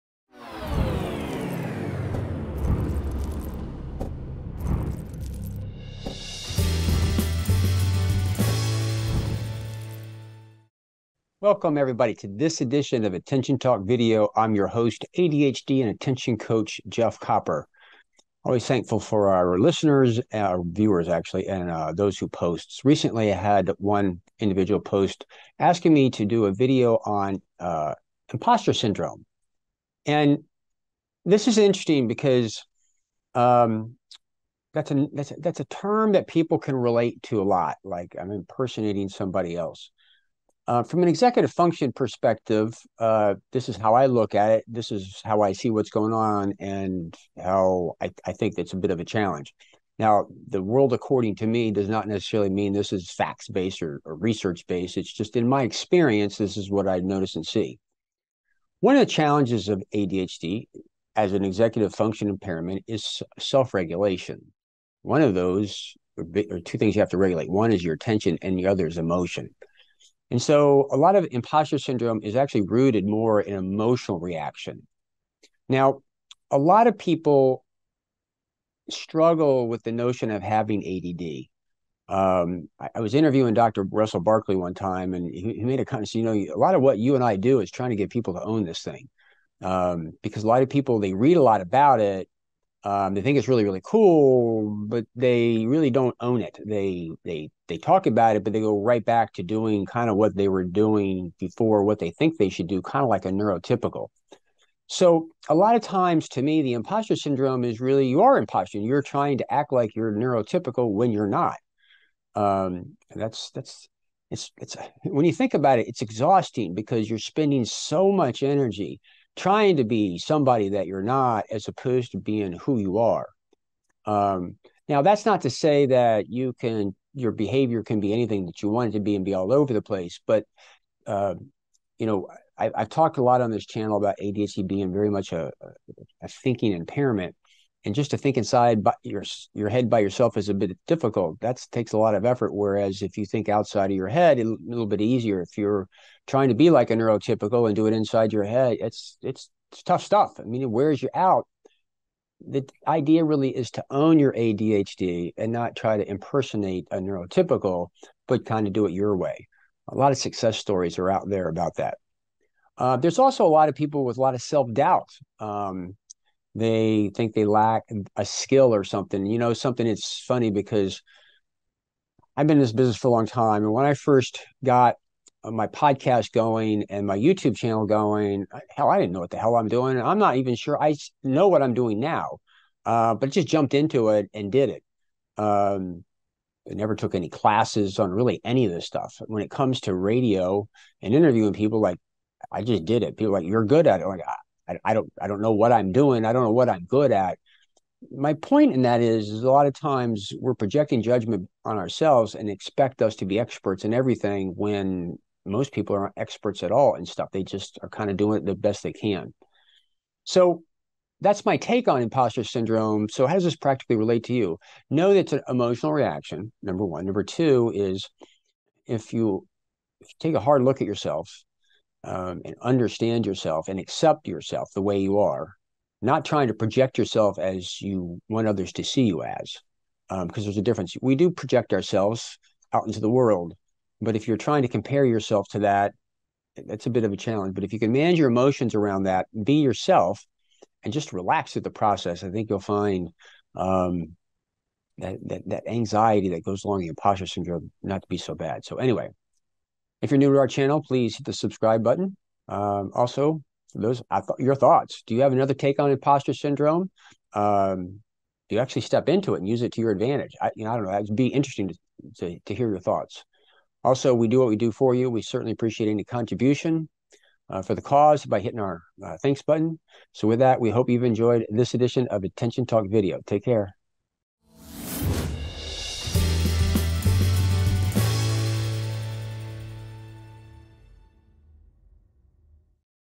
Impostor Syndrome and ADHD: Impersonating a Neurotypical?
This video is about Impostor Syndrome and ADHD: Impersonating a Neurotypical? How impostor syndrome can manifest in people with ADHD due to their self-regulation challenges.

The speaker, Jeff Copper, an ADHD and attention coach, also emphasizes that it's important to own your ADHD and not try to act like a neurotypical. The speaker also shares his own experience with impostor syndrome and encourages viewers to share their thoughts on the topic. Jeff shares his thoughts on what happens and why this is so difficult. If you struggle with ADHD, feel like an imposter, this is a video you won't want to miss.

Jeff Copper is an ADHD coach and host of Attention Talk Video which is part of the Attention Talk Network,